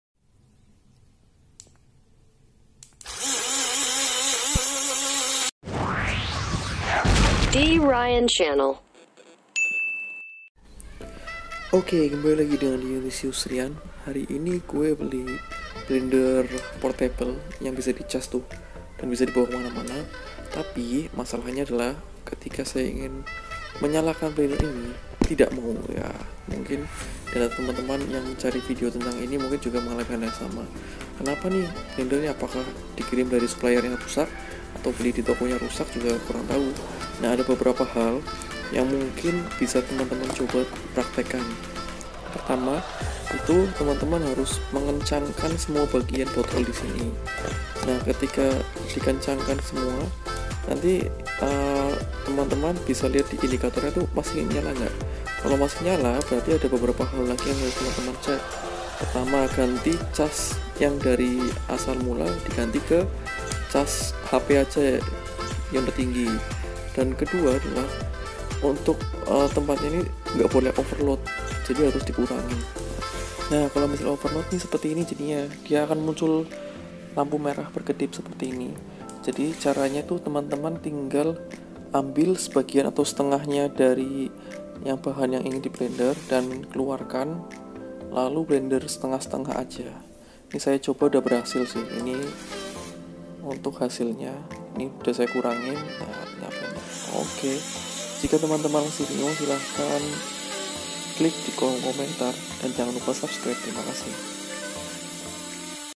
[JUICE CUP] NG-01 JUICE BLENDER PORTABLE, Baru beli gk bisa nyalaa ?? coba cara ini !
tutorial manual guide juice cup ng-01 buat ngeblender. cara nyalain, cara ngeblender pakai blender portabel.